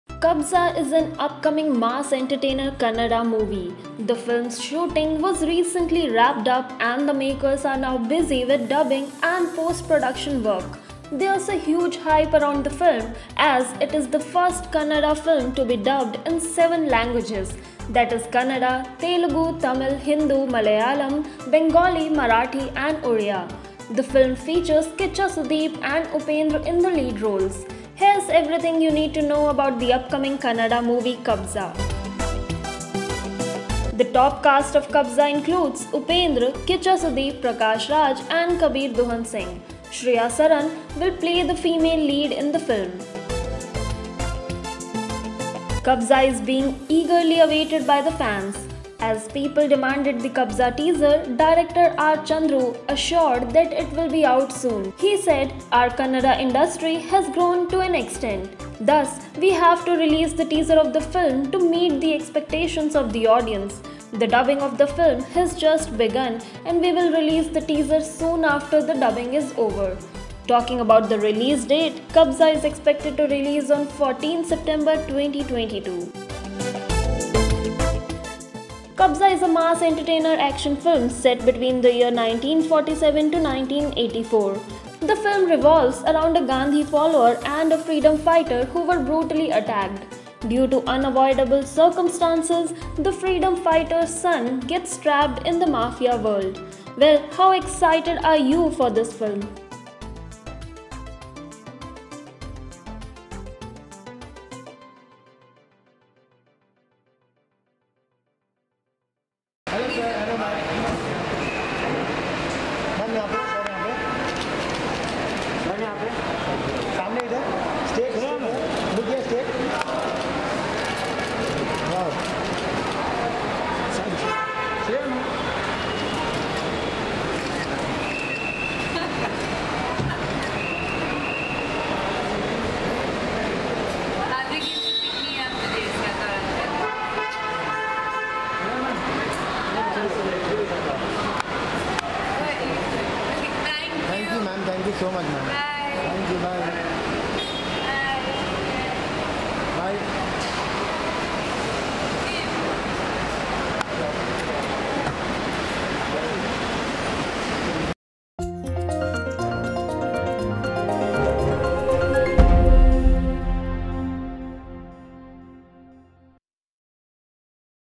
Kabza (Kannada Movie) Updates | Kiccha Sudeep, Upendra, Prakash Raj
Check out the latest updates on Kiccha Sudeep and Upendra starrer upcoming Kannada movie 'Kabza'.